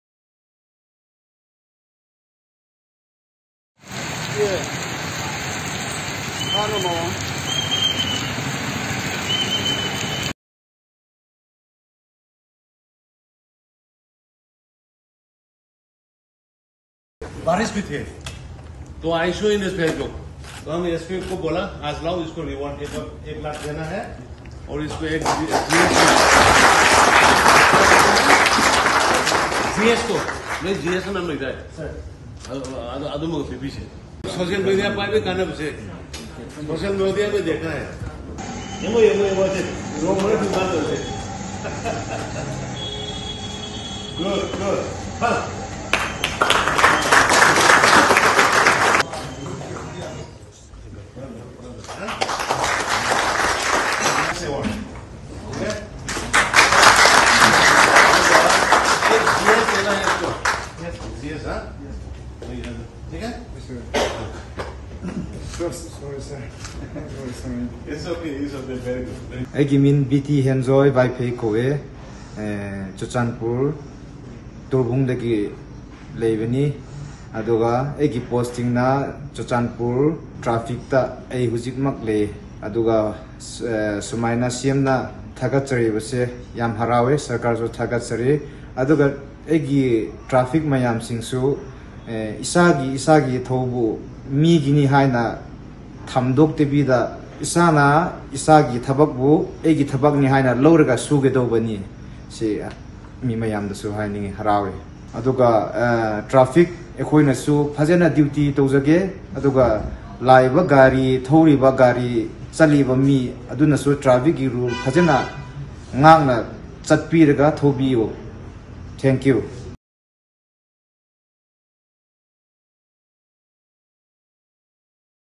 Hon'ble CM congratulated one Traffic policeman B.T. Henjoy Vaiphei of CCP Traffic Police Station
As given assurance by the Hon'ble CM N Biren Singh congratulated one Traffic policeman B.T. Henjoy Vaiphei of CCP Traffic Police Station and awarded a cash prize of Rs 1 lakh for his duty-bound and commitment to the safety of the people.

Published originally on May 11, 2022